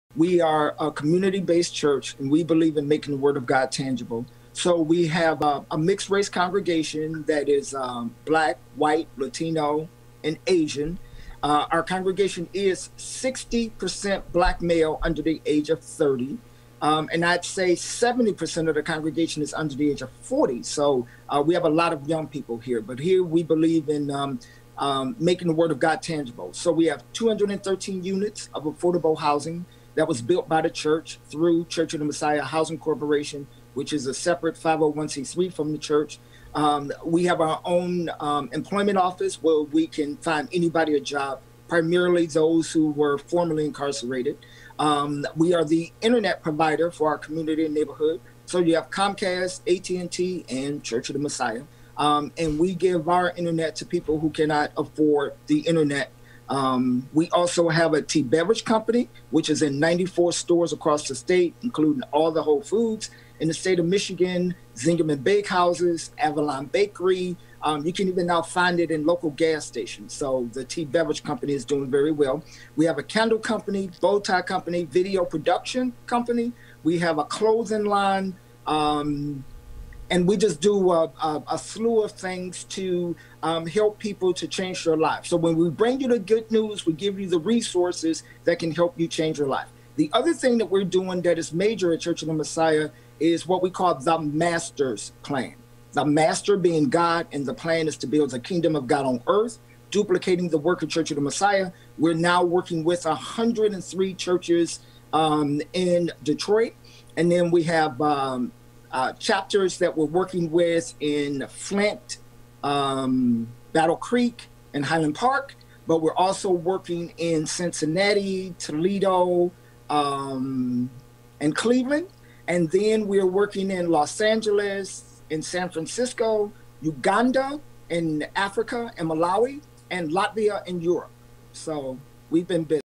Church of the Messiah Pastor Talks about Congregation & Details Many Services Provided by Community
Barry Randolph, Pastor at the Church of the Messiah, said the church emphasizes maintaining a tangible relationship with God, and makes that mission come to life by providing a number of services such as housing, job search aid, employment, internet services and more to people in need in the surrounding community!